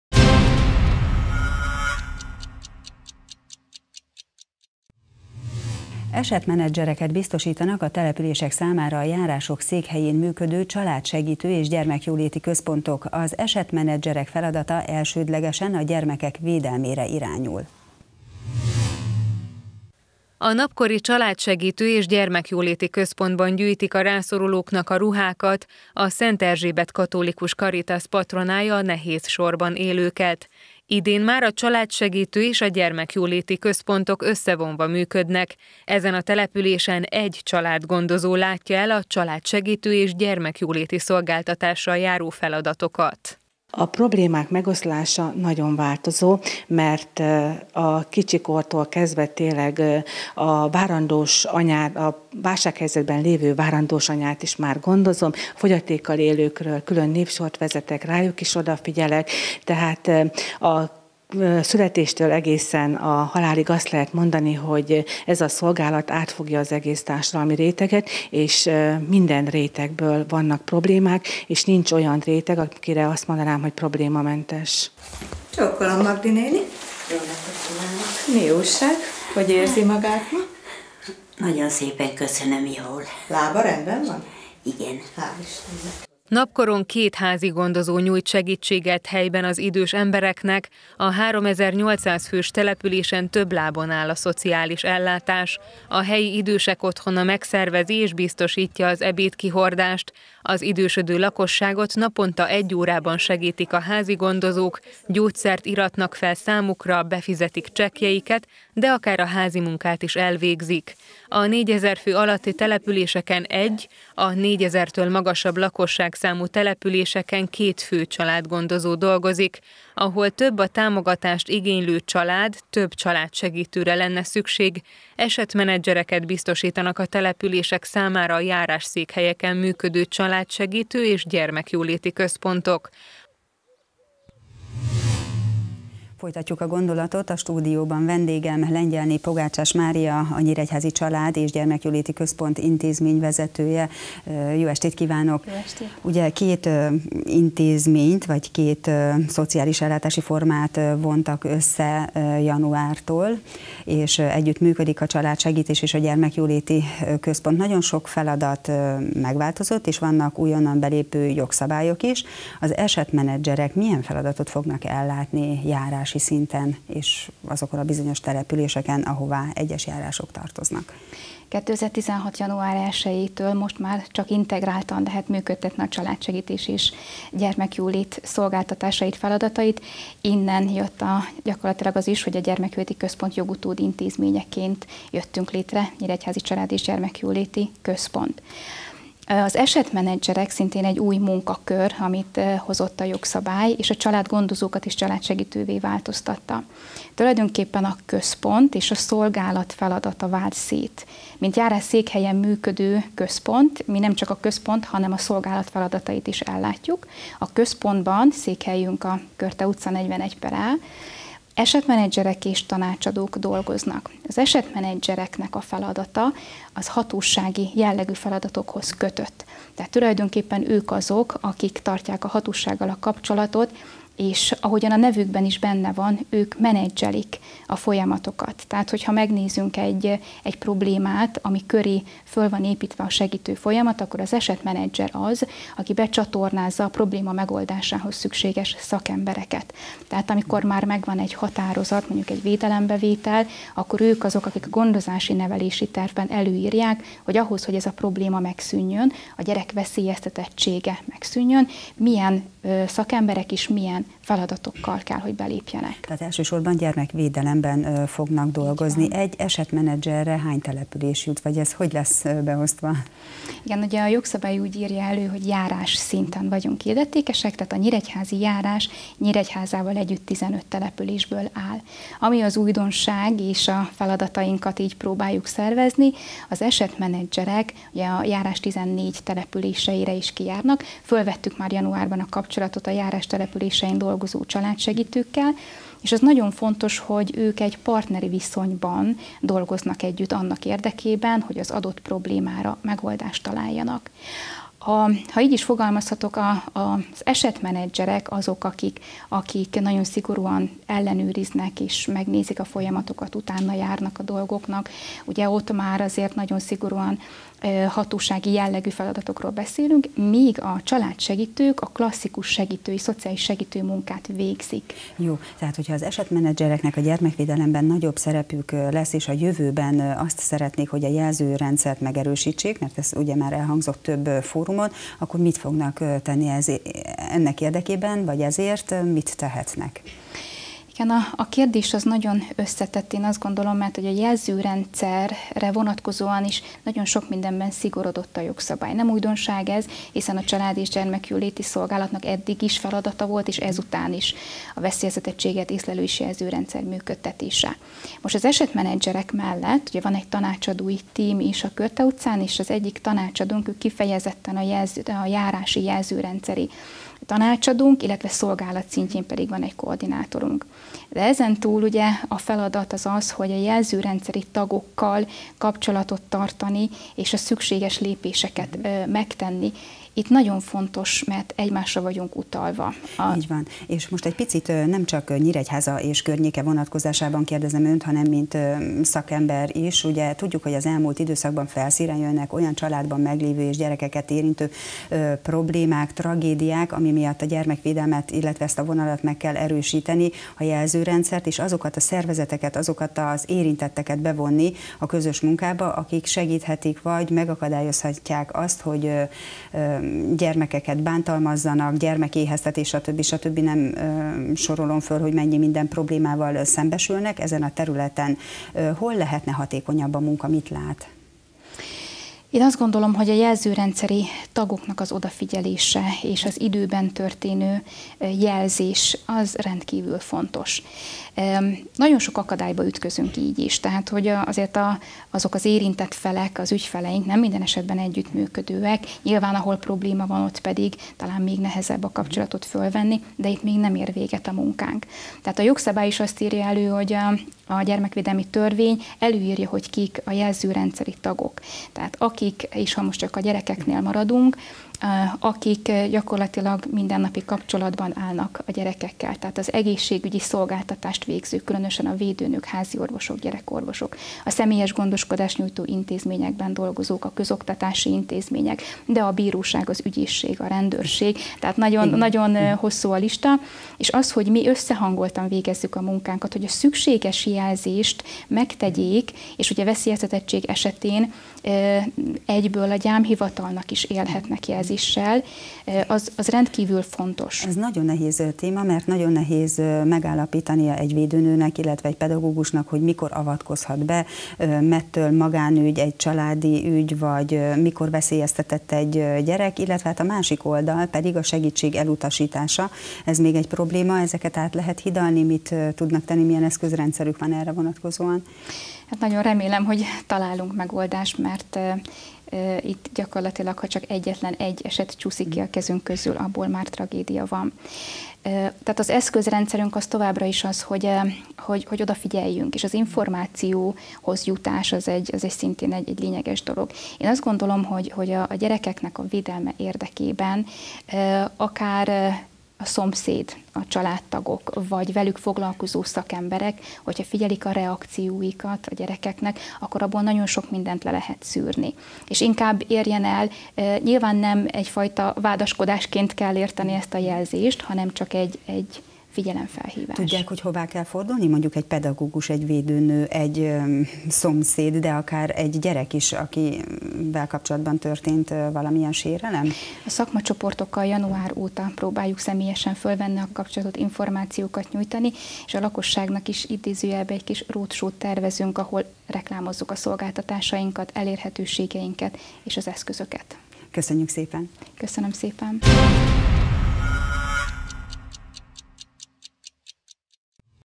Gyermekvédelem, veszélyeztetett gyermek a családban
Esetmenedzsereket biztosítanak a települések számára a járások székhelyén működő családsegítő-és gyermekjóléti központok. Az esetmenedzserek feladata  elsődlegesen a gyermekek védelmére irányul.